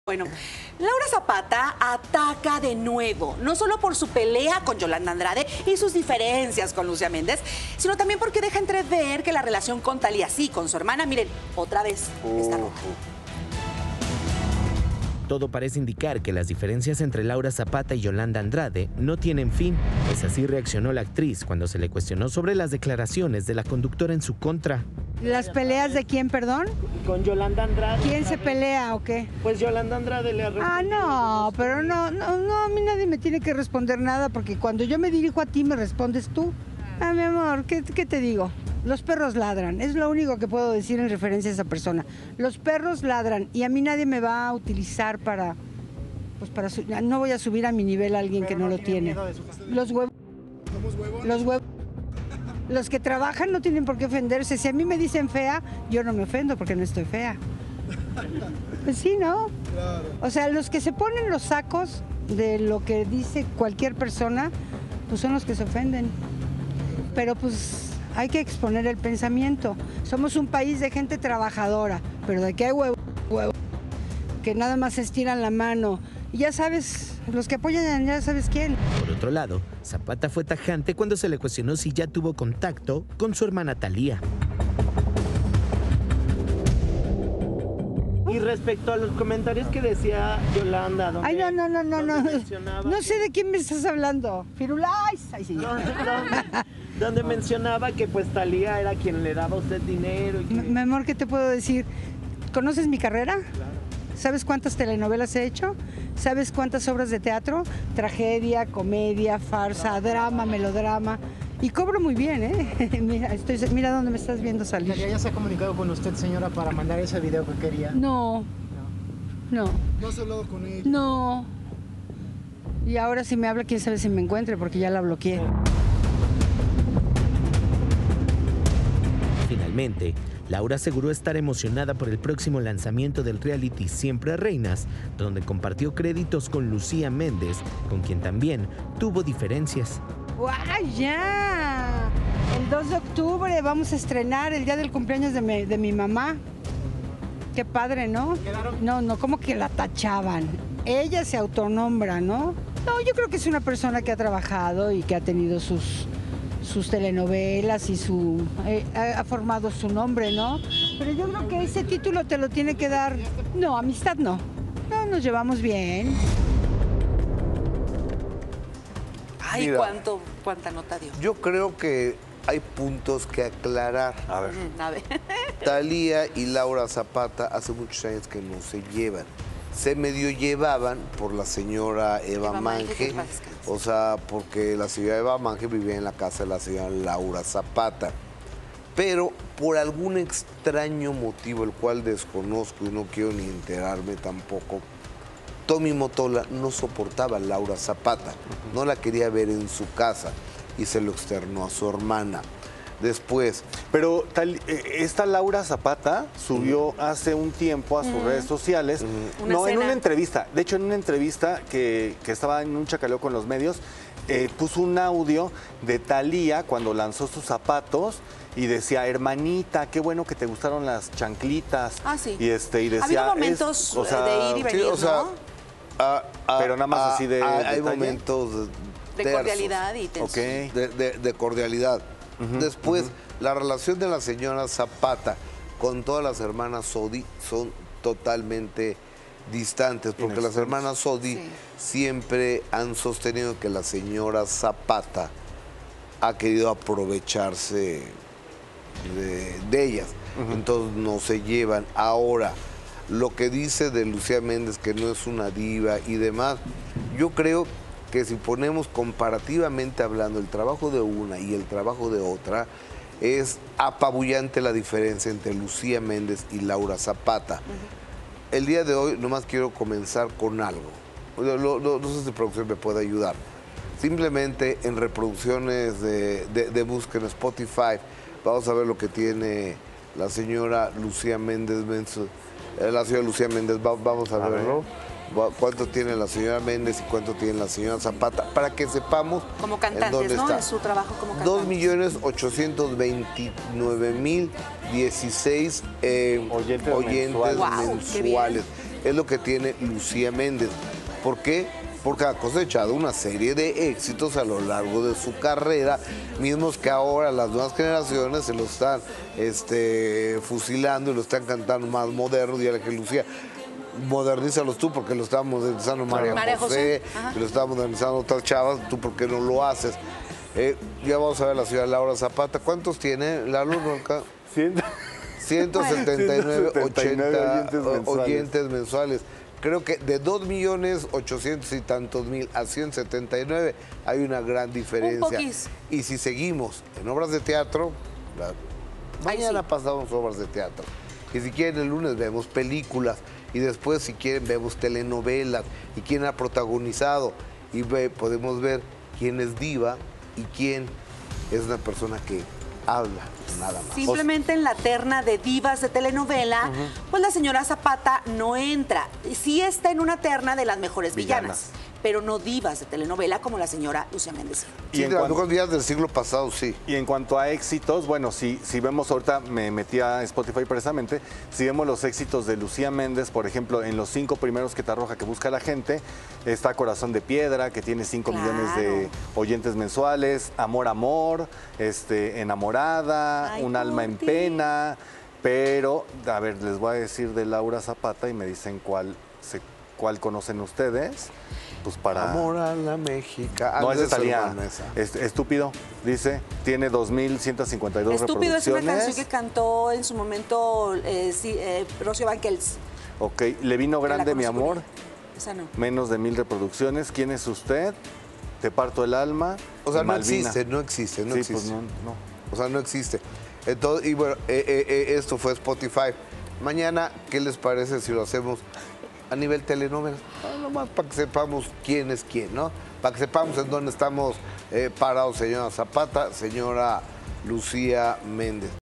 Laura Zapata ya bloqueó a Thalía de las redes sociales
La primera actriz, Laura Zapata, aseguró que los problemas con Yolanda Andrade no le dan importancia, pues declara que no pondrá su nivel al mismo que “a los perros”. Declaro que cuando Thalía intente buscarla será muy tarde, pues ya la tiene bloqueada.

27 de Septiembre 2022

COMENTA ESTE VÍDEO Y COMPÁRTELO CON TUS AMIGOS